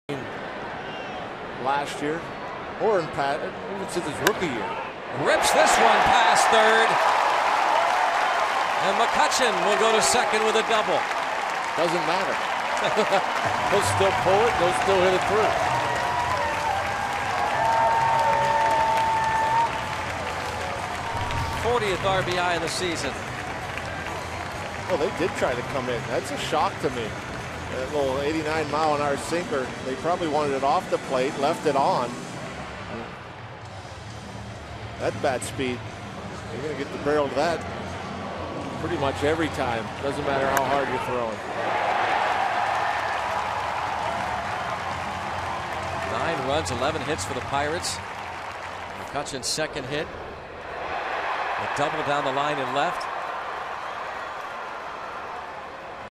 2013/06/28 McCutchen's RBI double
MIL@PIT: McCutchen smacks an RBI double to left